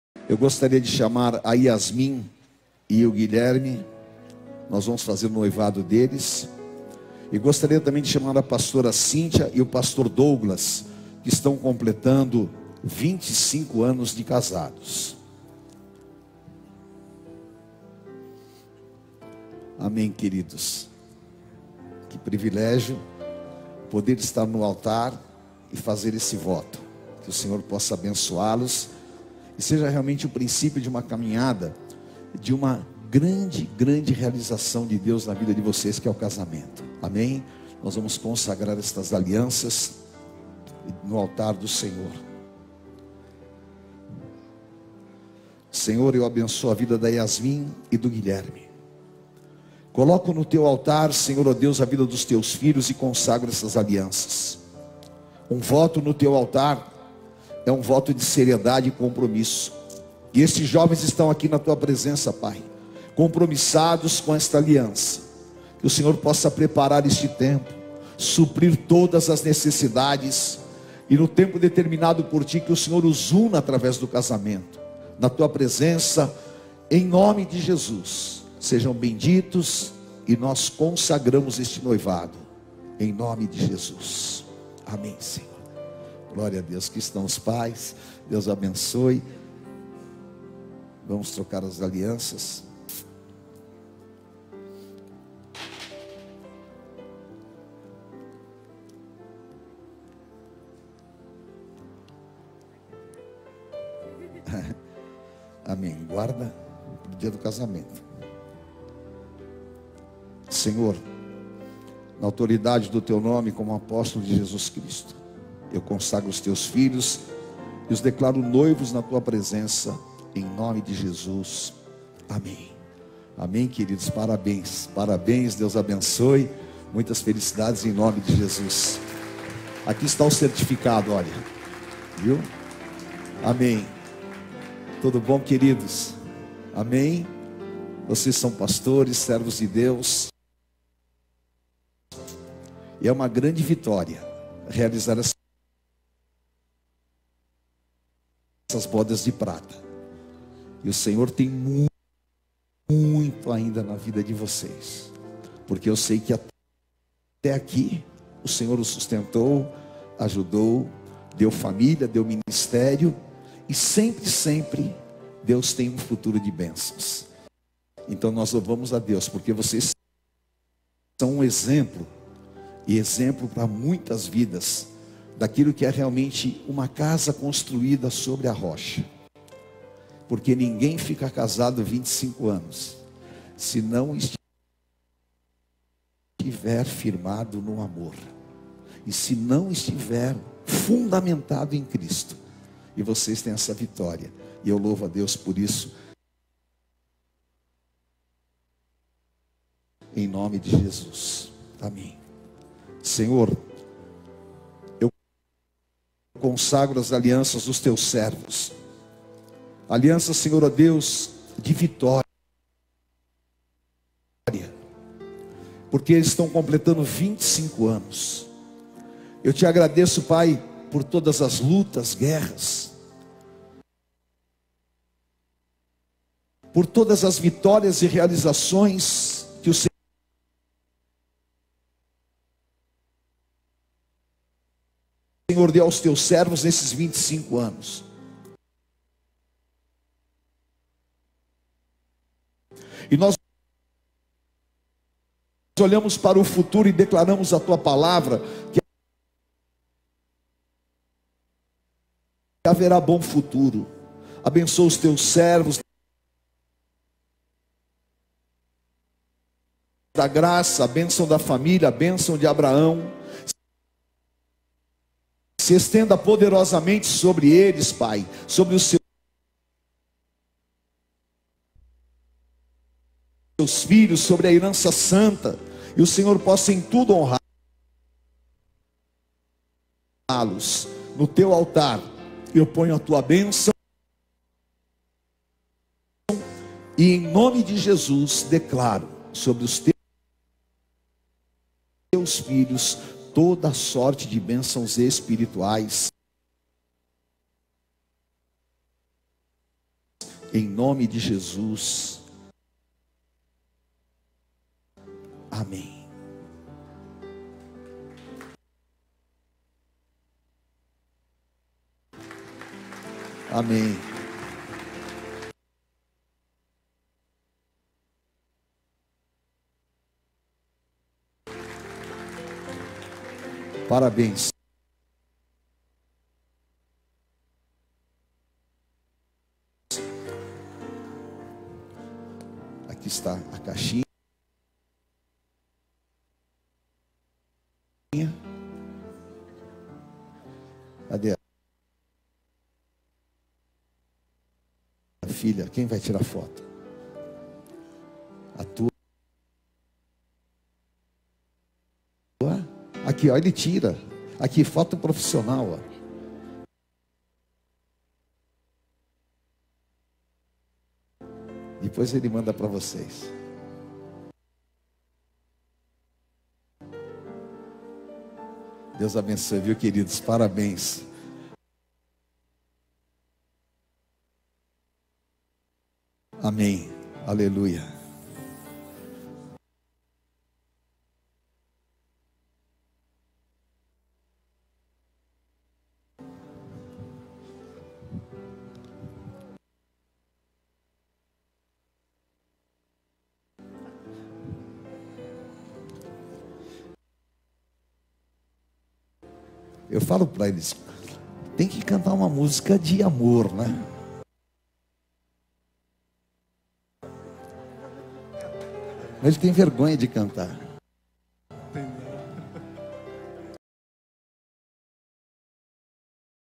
Pr.Douglas e Pra.Cintia Matavelli_Consagração de 25 anos (Bodas de prata)
Consagração de 25 anos (Bodas de prata) no
Renascer Hall Mooca 10/01/21
Com Ap.Estevam e Bpa.Sônia
Pr.Douglas Macknight e Pra.Cintia Matavelli